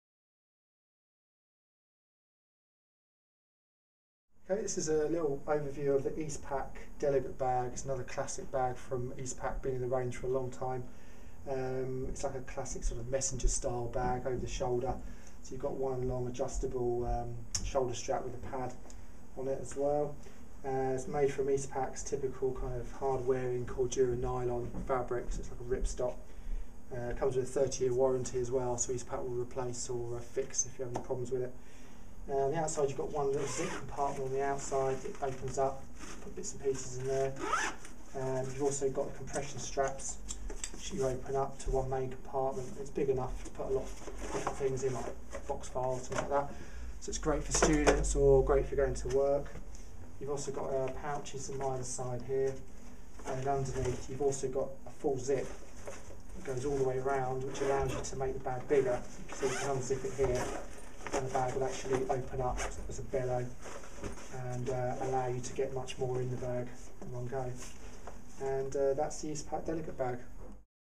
Eastpak Delegate Bag on Urbanindustry.com
Reflective zipper pull tabs * Rain covered zippers * Finished taped seams * Adjustable and detachable padded shoulder strap with soft grip system * Back outside velcro slash pocket * Two pockets under flap * Buckle closures on front flap * Expandable capacity * Size 40 x 31 x 13 cm * 22 Litres

Every Season we carry a wide selection of colours of the Eastpak delegae bag at Urbanindustry.com